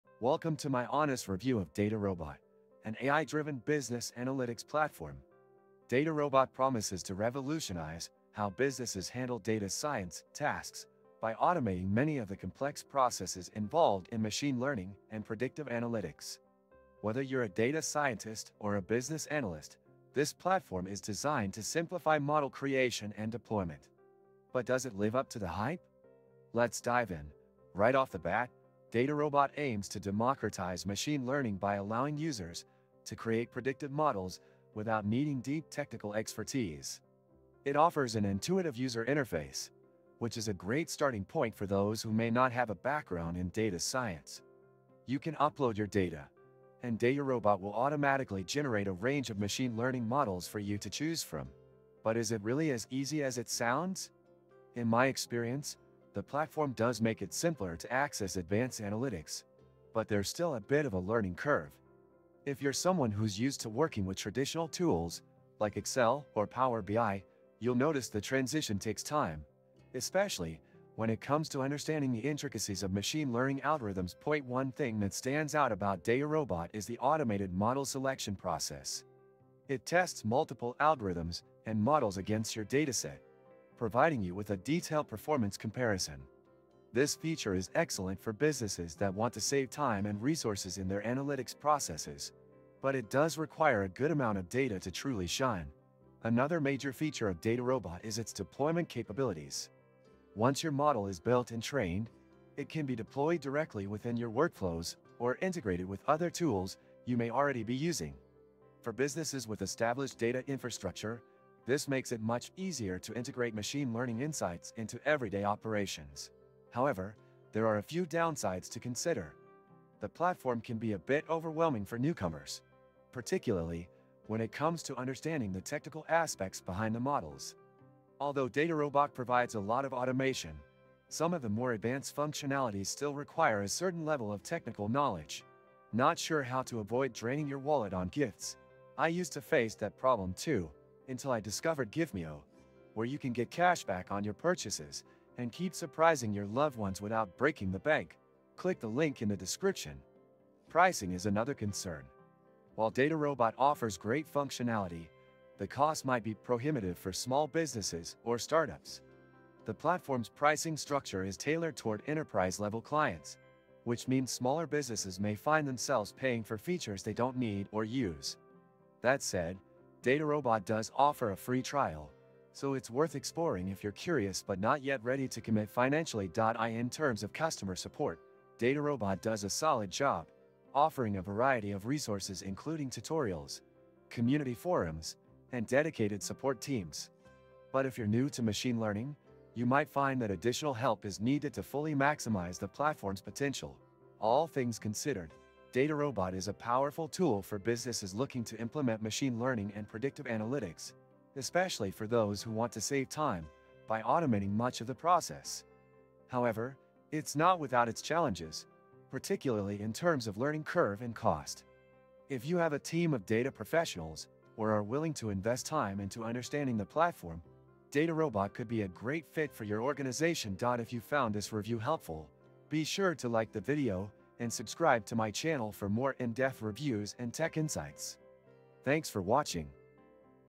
DataRobot: Honest Review of the AI-Powered Analytics Platform and My User Experience
In this video, I’ll walk you through my honest experience using DataRobot, an AI-powered business analytics platform designed to automate and streamline data science processes. From machine learning models to predictive analytics, this platform promises a lot. But does it live up to the hype? Watch my review for an in-depth look at its features, pros, and cons. - get cashback on your purchases!)